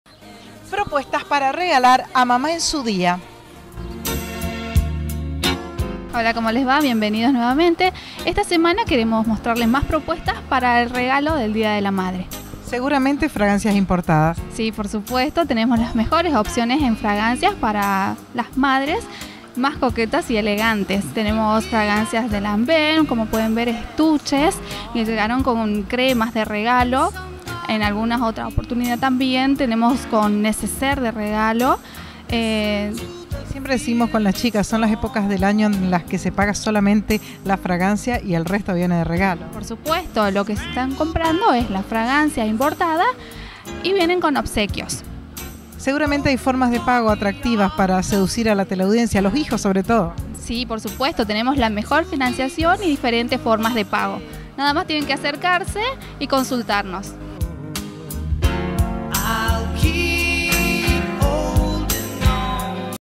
Peatonal Junín 937
Con Mercedes Sanchez seguimos viendo regalos para Mamá en su día . . .